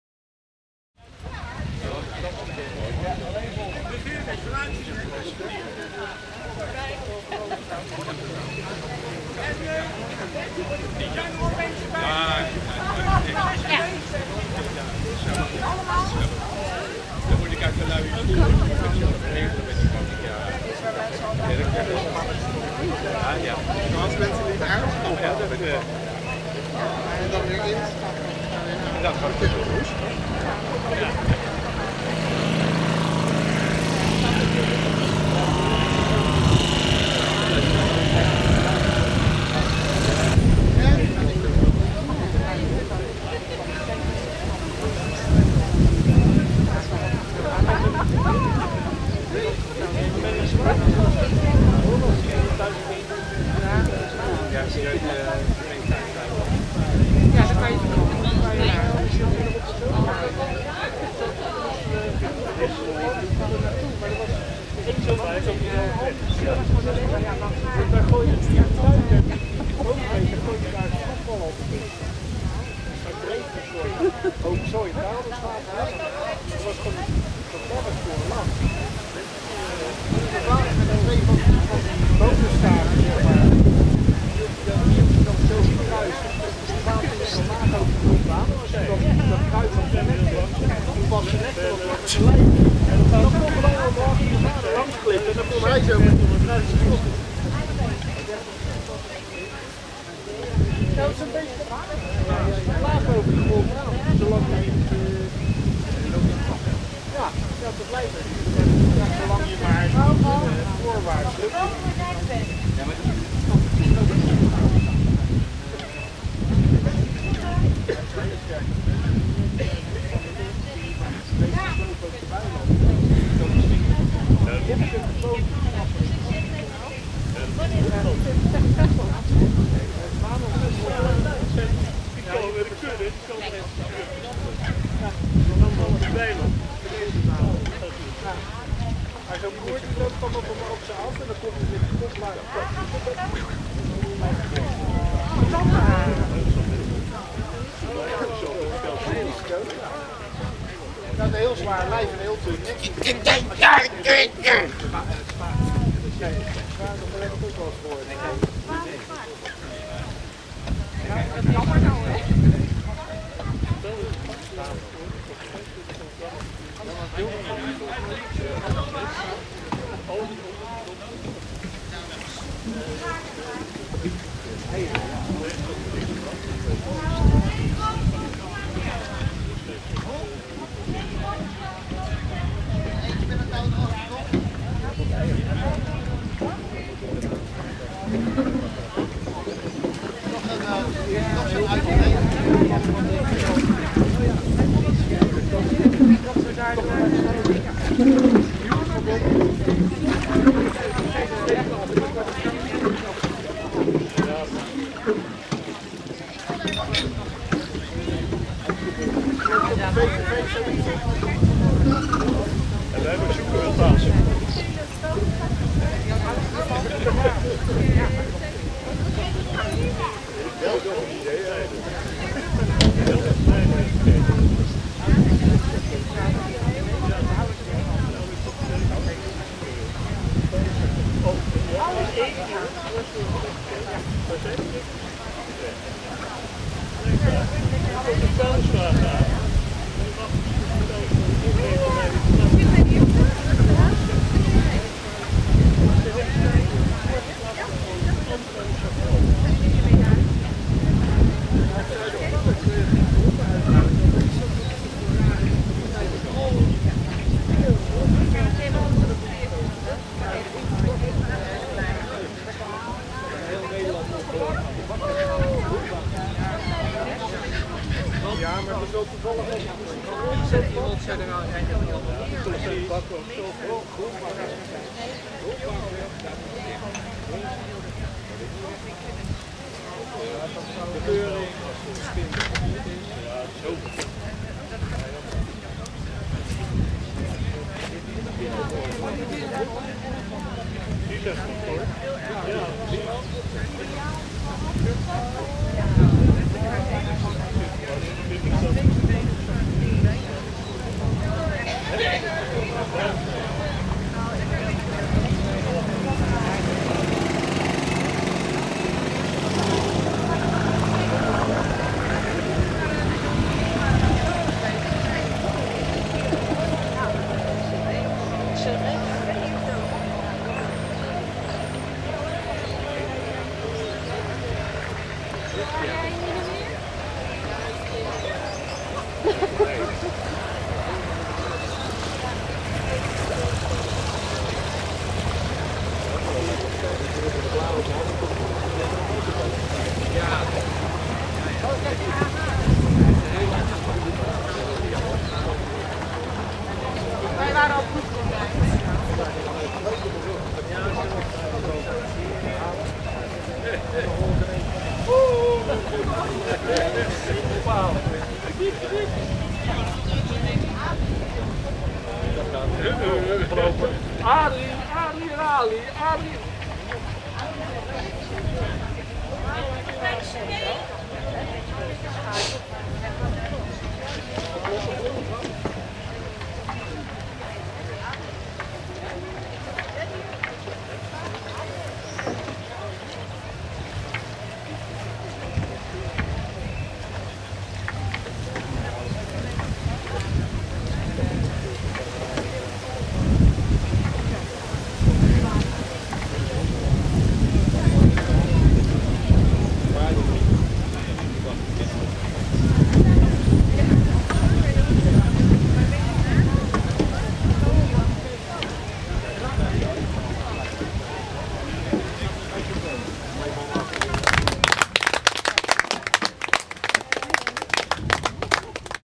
Jeugdtheaterschool Leiden
FESTIVAL ONDERUIT 2010
Op maandag 24 mei deed de Jeugdtheaterschool Leiden mee aan het festival.
Op een eilandje in het Rietveld in Hazerswoude speelde de Opleidingsklas Gevorderden een korte act, die gedurende de hele dag tweemaal per uur werd opgevoerd.